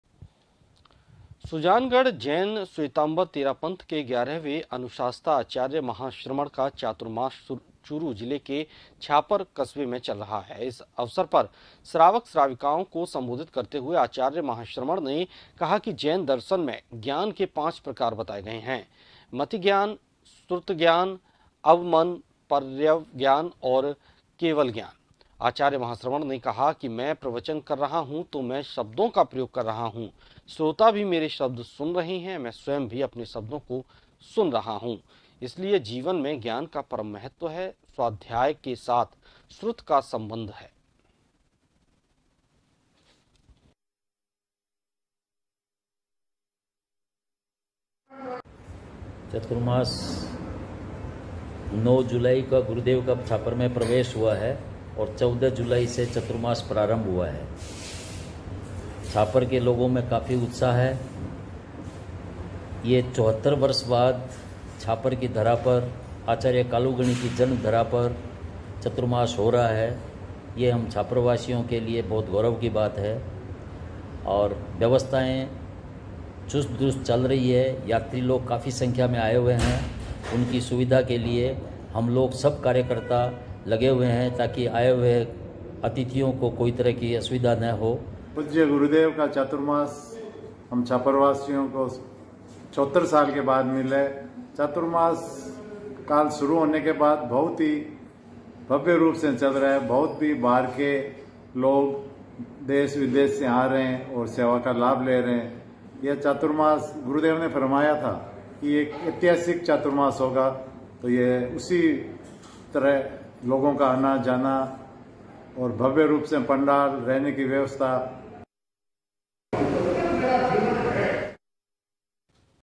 Churu जिले के छापर में Acharya Mahashraman का चातुर्मास, कहा- जीवन मे ज्ञान का परम महत्व
NEWS CREDIT-GS